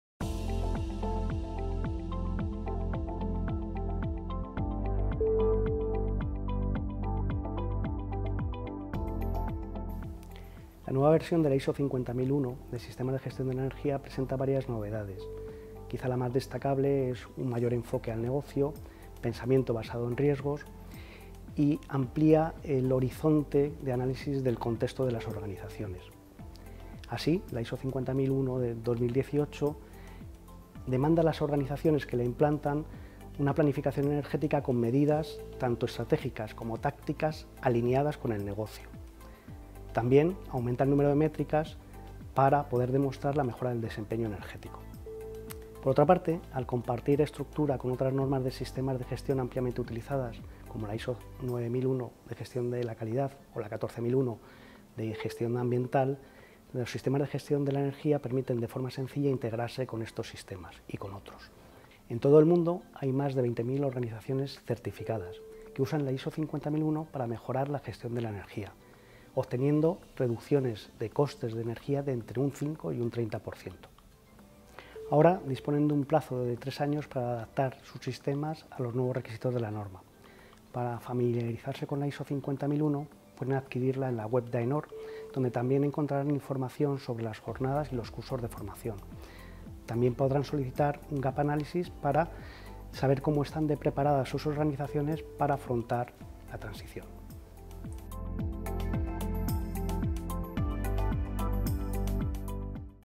Nueva ISO 50001 - Sistemas de gestión de la energía
La nueva versión de la ISO 5001 ya está disponible. Conoce qué novedades incopora y cómo puedes prepararte para adaptar tu sistema de gestión de la energía.